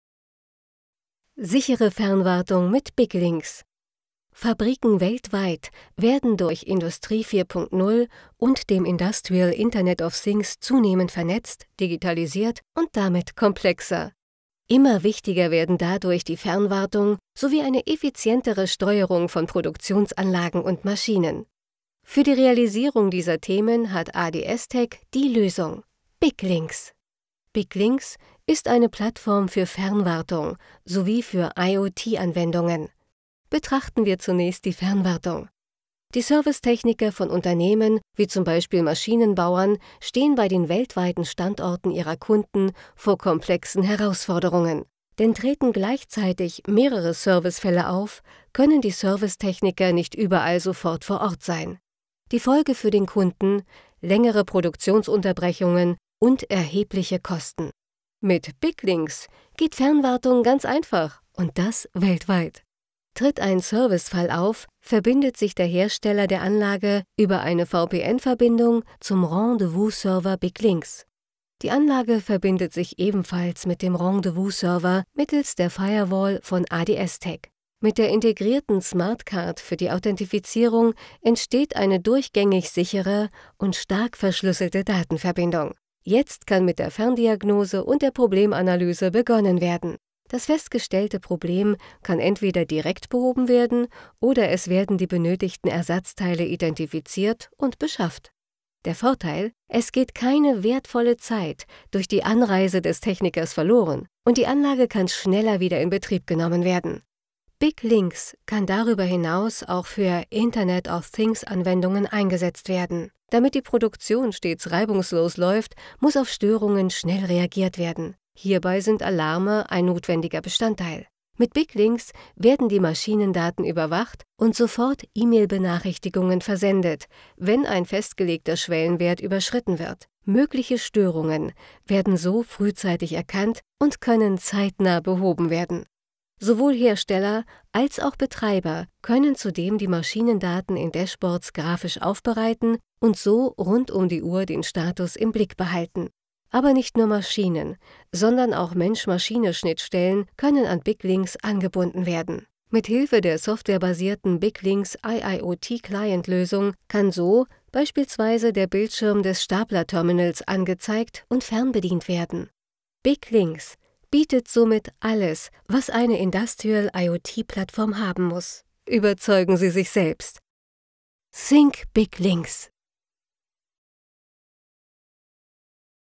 Big-LinX® IIoT Service & Maintenance Plattform – Basis für Fernwartung sowie IoT- Anwendungen
Mit Big‑LinX steht Herstellern und Betreibern von Maschinen & Anlagen, sowie Industrie 4.0 Dienstleistern eine vielseitige und skalierbare IIoT Service & Maintenance Plattform zur Verfügung. Der leistungsstarke Anwendungs-Baukasten bietet nicht nur eine einfache und sichere Fernwartung, sondern auch vielseitige IoT-Anforderungen können verwirklicht werden, um damit individuelle unternehmerische Ziele sicher zu realisieren. Technologie 100% Made in Germany.